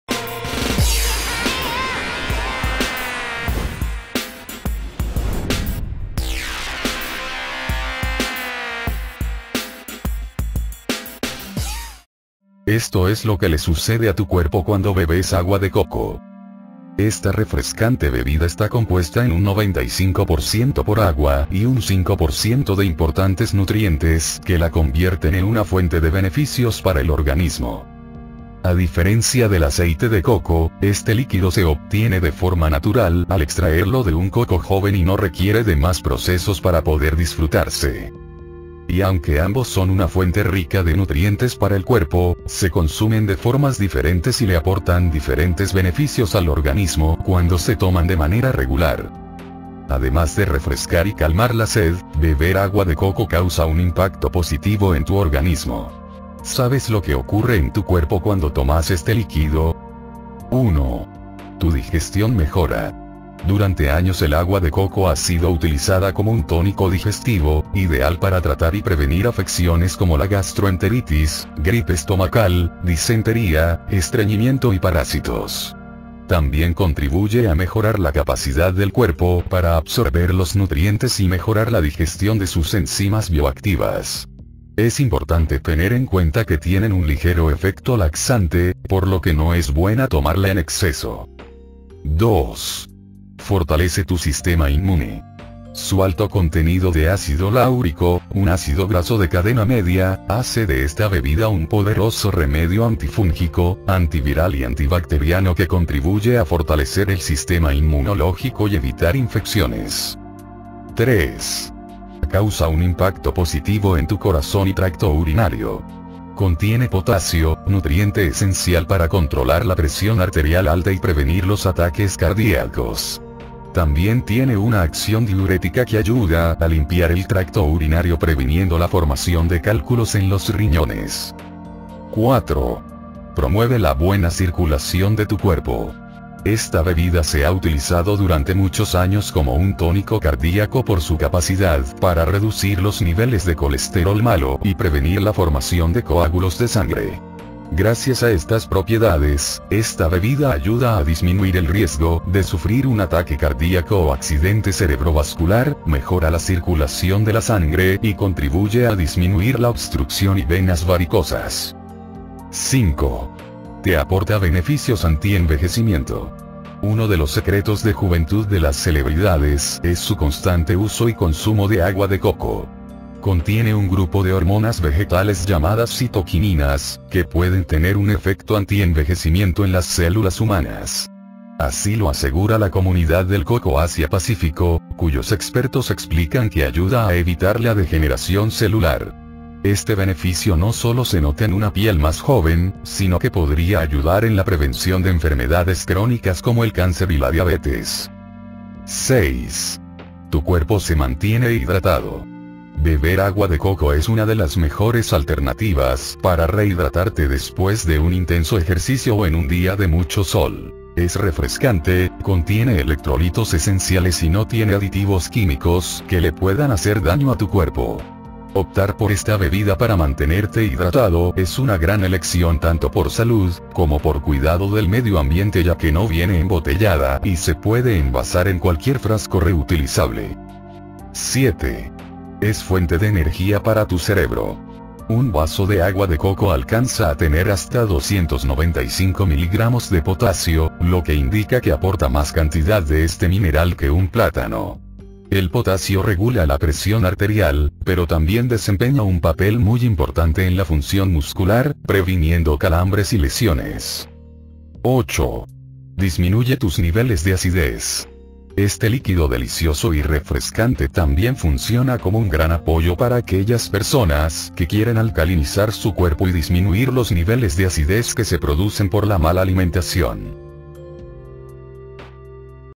Esto es lo que le sucede a tu cuerpo cuando bebes agua de coco
Esta refrescante bebida está compuesta en un 95%  por agua y un  5% de importantes nutrientes que la convierten en una fuente de beneficios para el organismo.

A diferencia del aceite de coco, este líquido se obtiene de forma natural al extraerlo de un coco joven y no requiere de más procesos para poder disfrutarse.

Y aunque ambos son una fuente rica de nutrientes para el cuerpo, se consumen de formas diferentes y le aportan diferentes beneficios al organismo cuando se toman de manera regular.

Además de refrescar  y  calmar la sed, beber agua de coco causa un impacto positivo en tu organismo. ¿Sabes lo que ocurre en tu cuerpo cuando tomas este líquido?

1. Tu digestión mejora.
Durante años el agua de coco ha sido utilizada como un tónico digestivo, ideal para tratar y prevenir afecciones como la gastroenteritis (gripe estomacal), disentería, estreñimiento y parásitos.
También contribuye a mejorar la capacidad del cuerpo para absorber los nutrientes y mejorar la digestión de sus enzimas bioactivas.
Es importante tener en cuenta que tienen un ligero efecto laxante, por lo que no es buena tomarla en exceso.

2. Fortalece tu sistema inmune.
Su alto contenido de ácido láurico,  un ácido graso de cadena media— hace de esta bebida un poderoso remedio antifúngico, antiviral y antibacteriano que contribuye a fortalecer el sistema inmunológico y evitar infecciones.

3. Causa un impacto positivo en tu corazón y tracto urinario.
Contiene potasio, nutriente esencial para controlar la presión arterial alta y prevenir los ataques cardíacos.
También tiene una acción diurética que ayuda a limpiar el tracto urinario previniendo la formación de cálculos en los riñones.

4. Promueve la buena circulación de tu cuerpo.
Esta bebida se ha utilizado durante muchos años como un tónico cardíaco por su capacidad para reducir los niveles de colesterol malo y prevenir la formación de coágulos de sangre.
Gracias a estas propiedades, esta bebida ayuda a disminuir el riesgo de sufrir un ataque cardíaco o accidente cerebrovascular, mejora la circulación de la sangre y contribuye a disminuir la obstrucción y venas varicosas.

5. Te aporta beneficios antienvejecimiento.
Uno de los secretos de juventud de las celebridades es su constante uso y consumo de agua de coco.
Contiene un grupo de hormonas vegetales llamadas citoquininas, que pueden tener un efecto antienvejecimiento en las células humanas.  Así lo asegura la Comunidad del Coco Asia Pacífico, cuyos expertos explican que ayuda a evitar la degeneración celular.
Este beneficio no solo se nota en una piel más joven, sino que podría ayudar en la prevención de enfermedades crónicas como el cáncer y la diabetes.

6. Tu cuerpo se mantiene hidratado.
Beber agua de coco es una de las mejores alternativas para rehidratarte después de un intenso ejercicio o en un día de mucho sol: es refrescante, contiene electrolitos esenciales y no tiene aditivos químicos que le puedan hacer daño a tu cuerpo.
Optar por esta bebida para mantenerte hidratado es una gran elección tanto por salud, como por cuidado del medio ambiente ya que no viene embotellada y se puede envasar en cualquier frasco reutilizable.

7. Es fuente de energía para tu cerebro.
Un vaso de agua de coco alcanza a tener hasta 295 miligramos de potasio, lo que indica que aporta más cantidad de este mineral que un plátano.
El potasio regula la presión arterial, pero también desempeña un papel muy importante en la función muscular, previniendo calambres y lesiones.

8. Disminuye tus niveles de acidez.
Este líquido delicioso y refrescante también funciona como un gran apoyo para aquellas personas que quieren alcalinizar su cuerpo y disminuir los niveles de acidez que se producen por la mala alimentación.